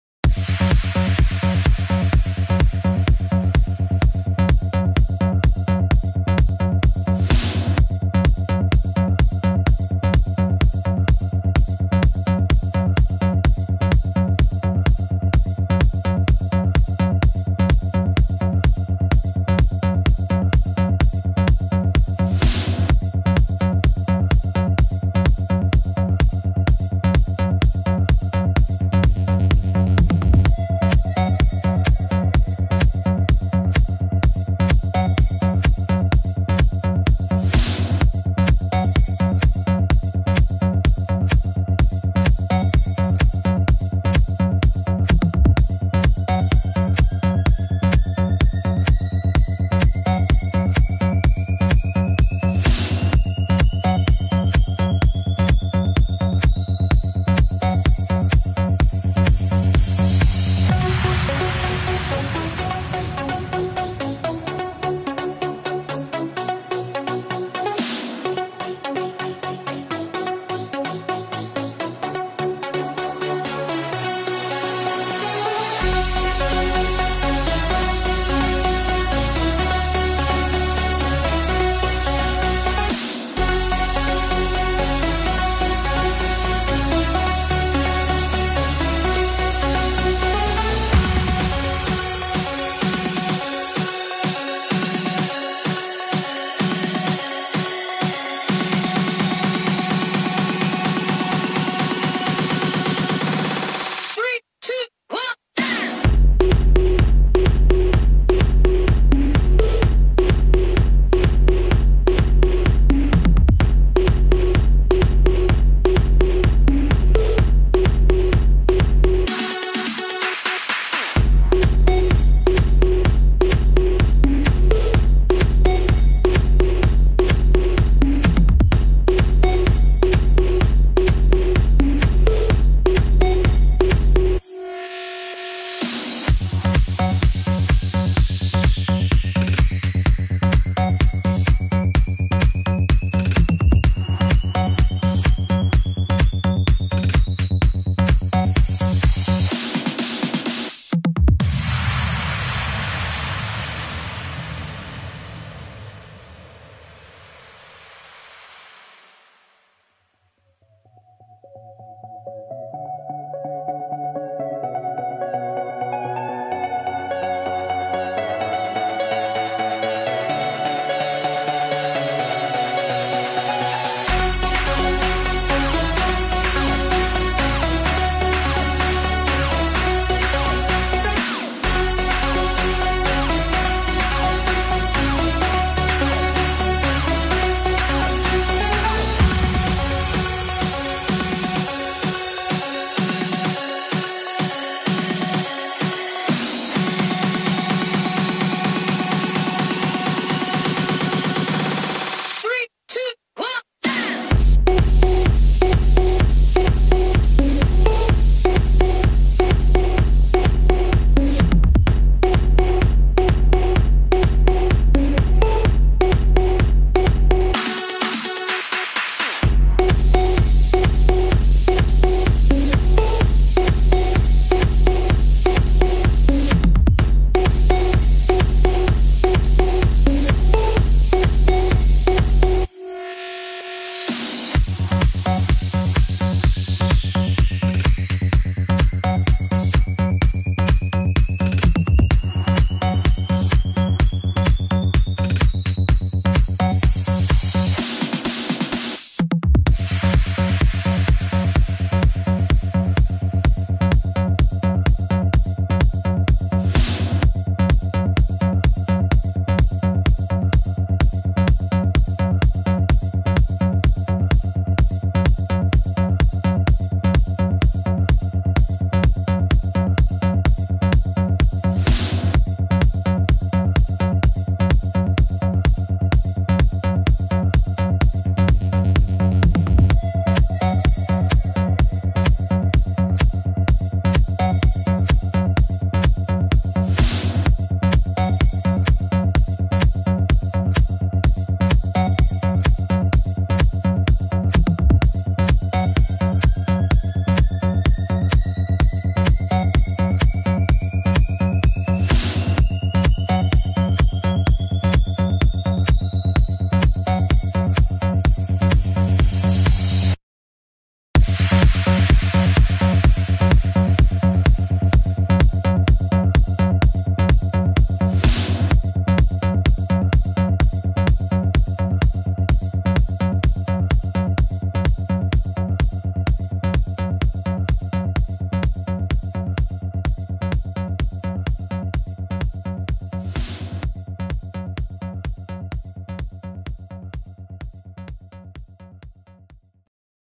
IMIKIMI Animated Star & Starry Photo Frames by Photo Fun and Art
IMIKIMI Animated Star & Starry Photo Frames.

Here we have more then 500 animated Star & Starry Photo frames for you.

You can create them with your own photo's on the imikimi site and surprise your Friends and with it.

Search for: starry animated on the IMIKIMI site ( on top right corner )

Watch the inspiration video.